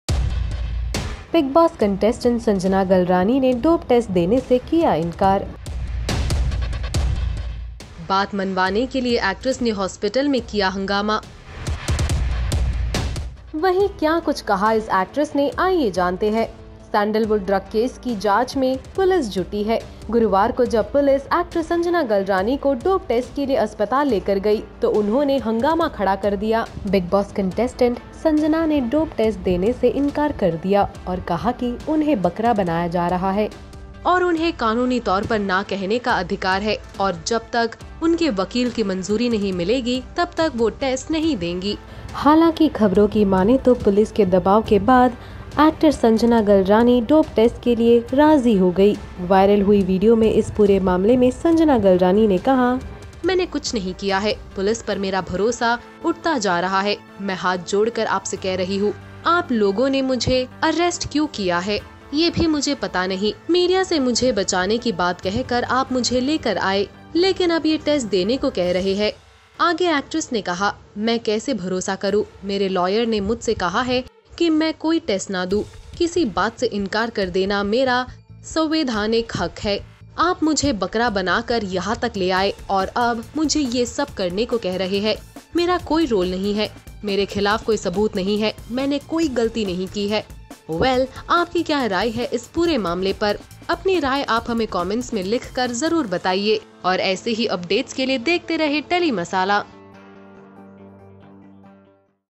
Paras Chhabra's Mujhse Shaadi Kaorge Show Contestant Sanjjanaa Galrani REFUSES Dope Test
Paras Chhabra and Shehnaaz Gill’s Mujhse Shaadi Karoge co-contestant Sanjjanaa Galrani dramatically refuses to give blood samples for dope test.  Sanjjaana is Also Well Known Actress In South & Done Many Projects. Watch Video To Know More.